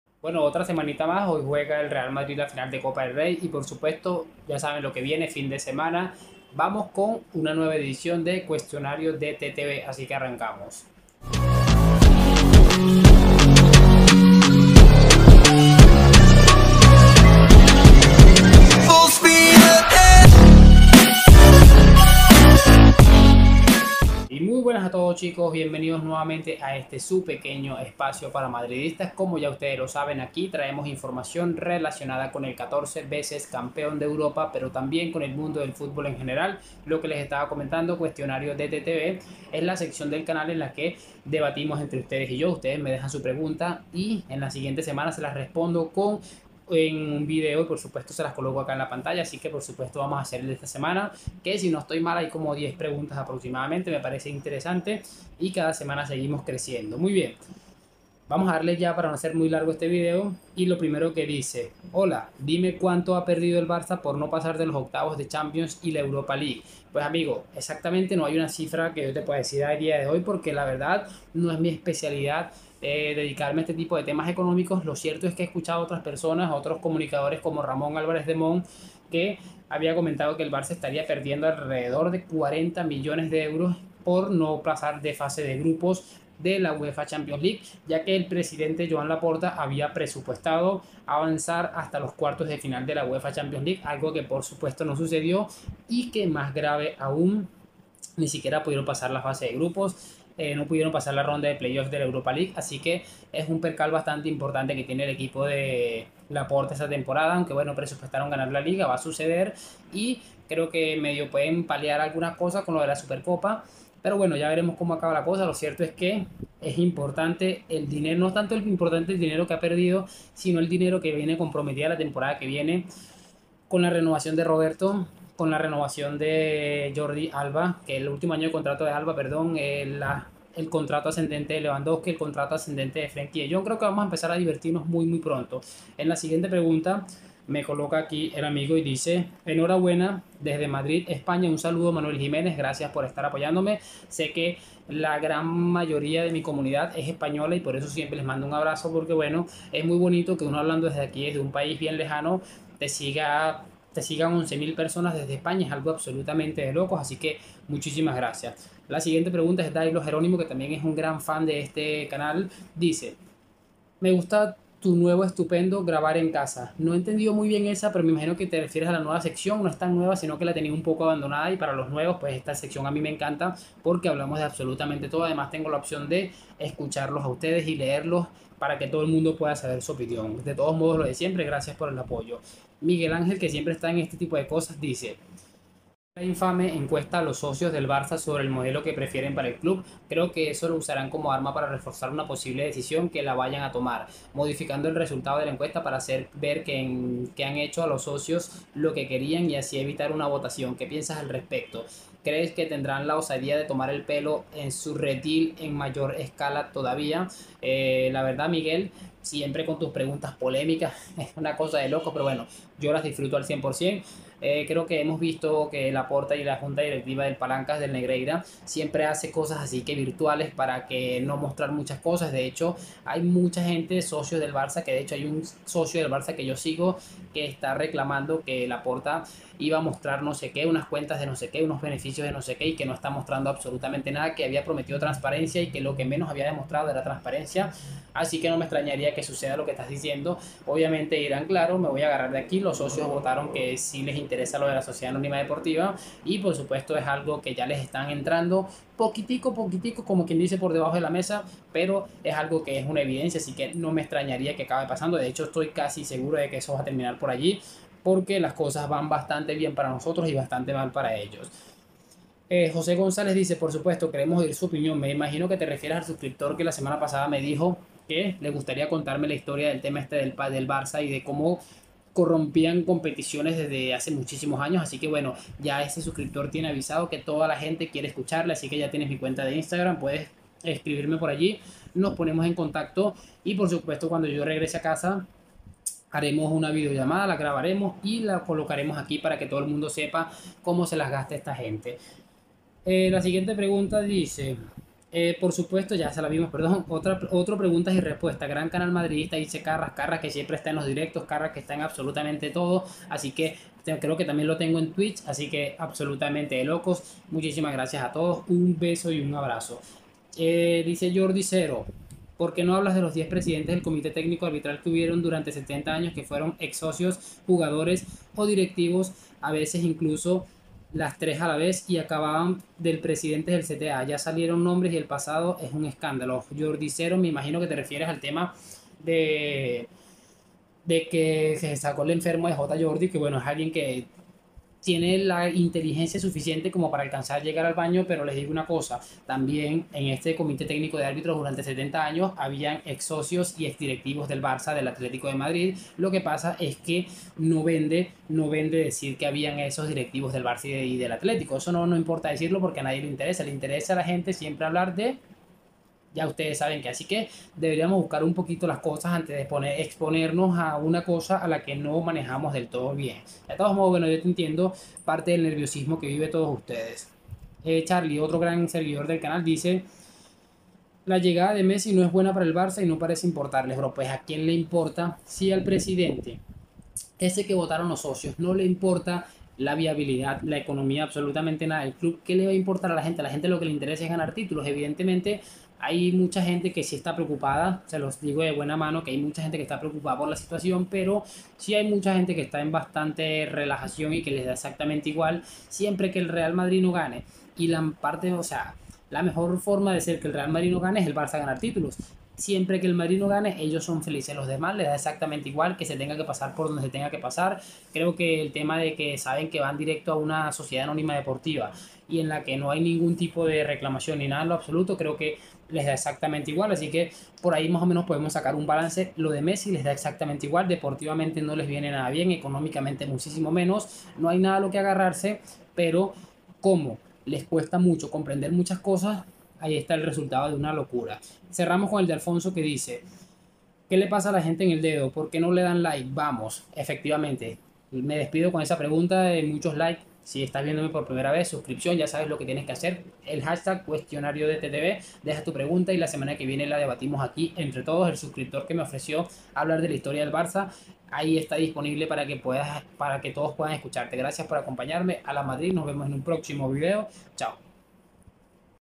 BARÇA SERÁ SOCIEDAD ANÓNIMA SEGURO | CTA SOCIOS DEL MADRID 70 AÑOS | LA VUELTA DE MESSI BARCELONA
Cuestionario DTTV. Es mi sección favorita del canal, donde la dinámica es la siguiente: ustedes dejan en los comentarios una pregunta y yo se las respondo la semana siguiente con un vídeo. No importa que clase de preguntas, todas son bienvenidas. Sólo debes escribirla con el hashtag cuestionariodttv.

En el de esta semana tenemos preguntas interesantes cómo:

-CASO NEGREIRA BARCELONA

-VUELTA DE MESSI AL BARÇA

- PODRIDO DEL FÚTBOL ESPAÑOL.

-LAPORTA, REAL MADRID, ETC.

Y otras tantas.

¡No olvides participar! SÓLO TIENES QUE DEJAR TU COMENTARIO CON EL HASTAG